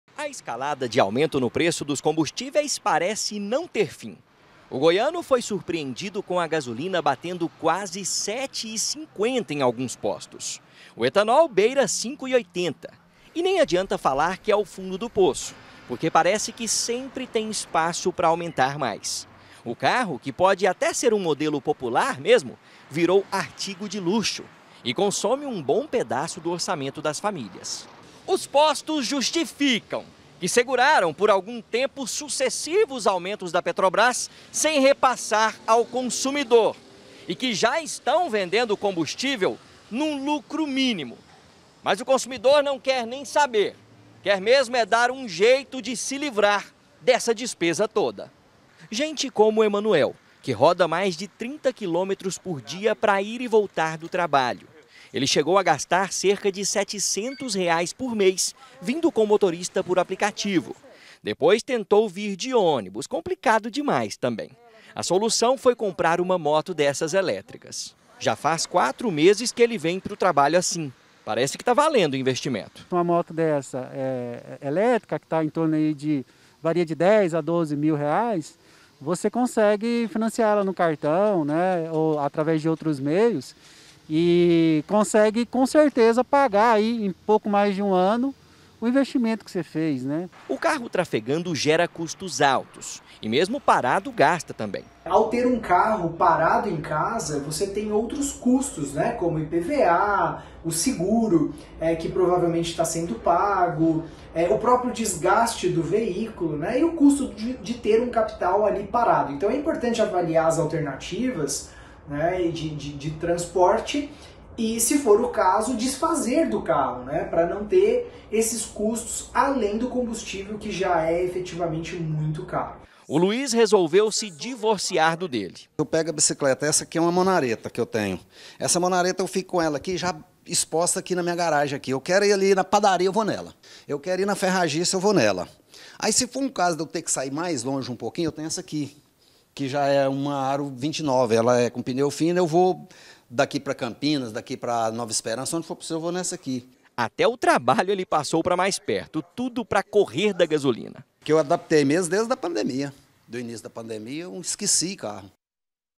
AINDA MAIS CARA: COMO FUGIR DO AUMENTO DA GASOLINA E ECONOMIZAR
FOI SÓ PASSAR O FERIADO QUE O PREÇO DOS COMBUSTÍVEIS VOLTOU A SUBIR.